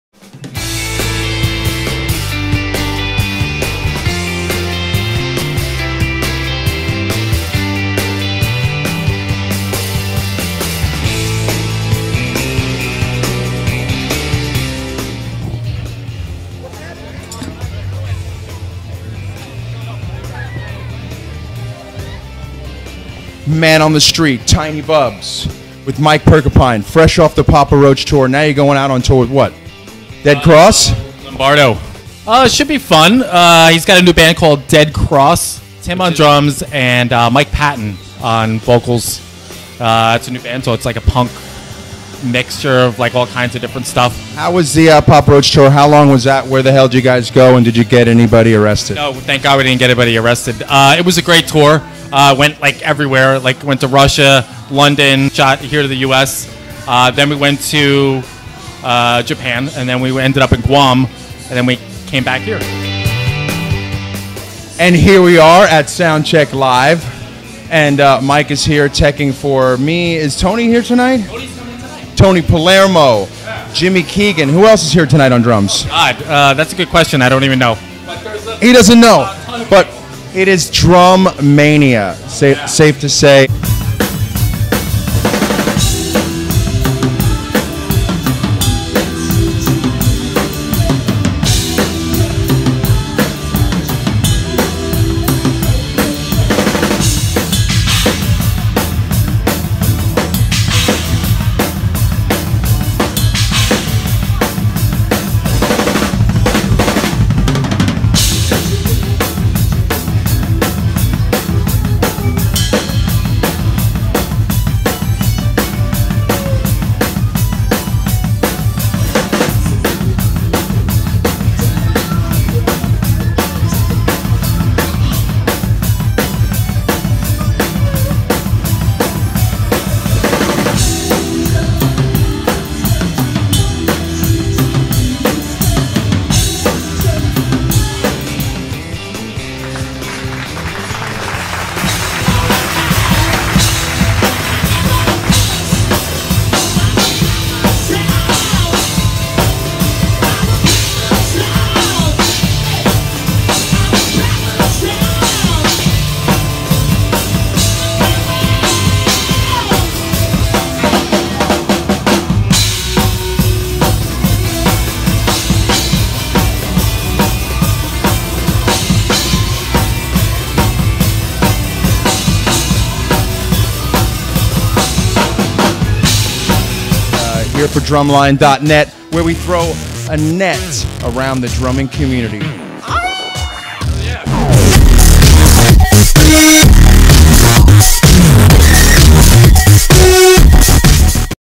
Drum-Line Tiny Bubz interviews Mike Perkinpine
Drum tech for Papa Roach/Dead Cross/Last In Line
Playing clips from Jimmy Keegan and Tawny Palermo